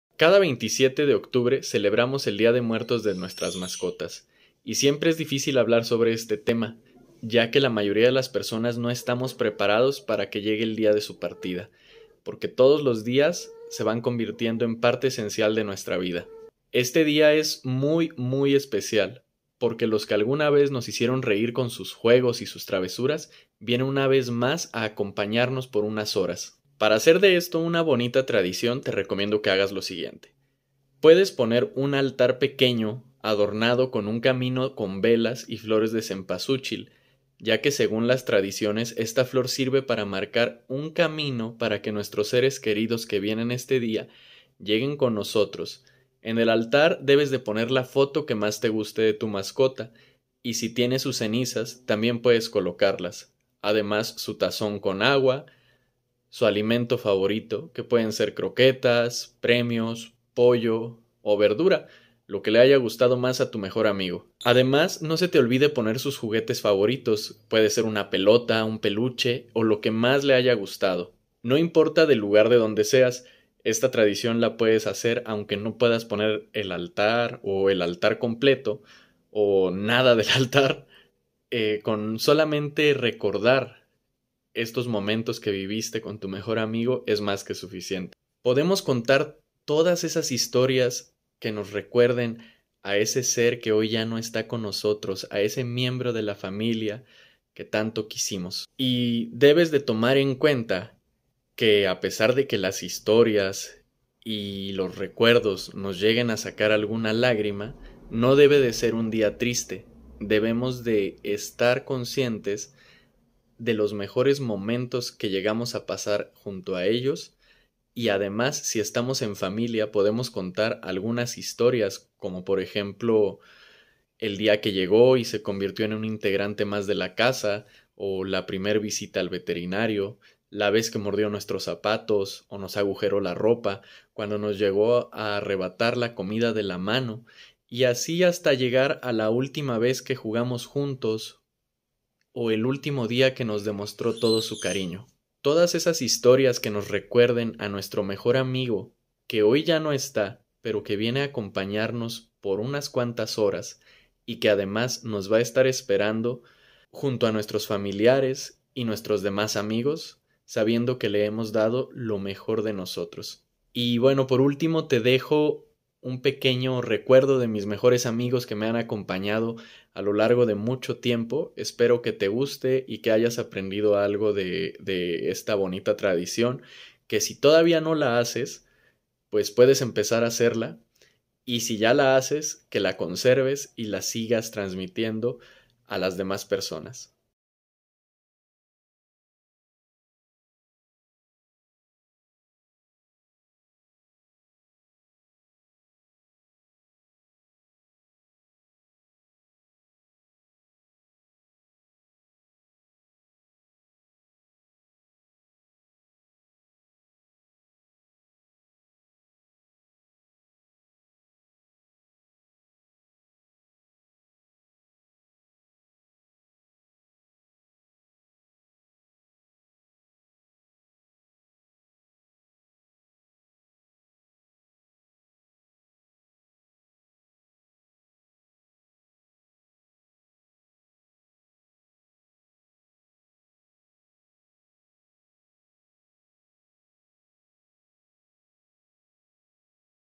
Dia de muertos para mascotas
En este video te cuento sobre el día de muertos, una festividad mexicana que consiste en recordar a nuestros seres queridos que ya no están con nosotros, pero enfocándolo en nuestros mejores amigos o mascotas.
El 27 de octubre de cada año se conmemora esta fecha para que cada año recordemos a nuestras mascotas.